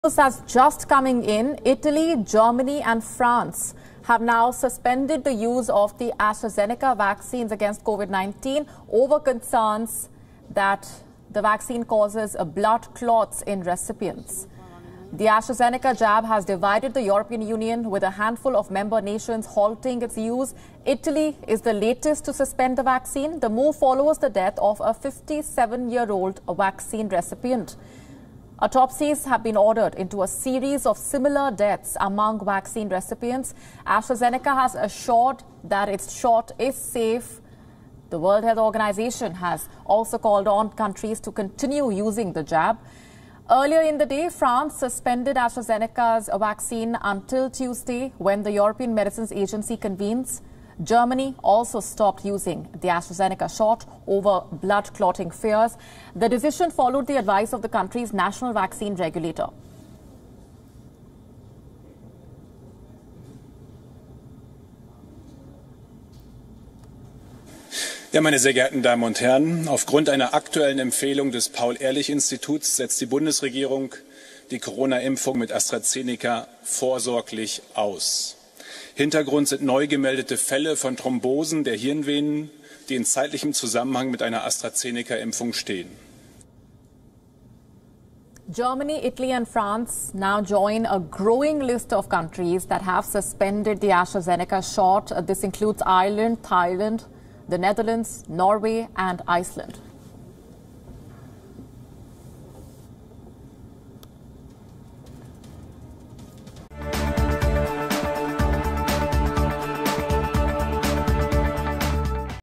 News Alert: Italy, Germany, France suspends AstraZeneca jabs | COVID-19 Vaccine | English News |WION
France, Italy and Germany became the latest major European countries to block Astrazeneca's Covid-19 vaccines over reports of dangerous blood clots in some recipients.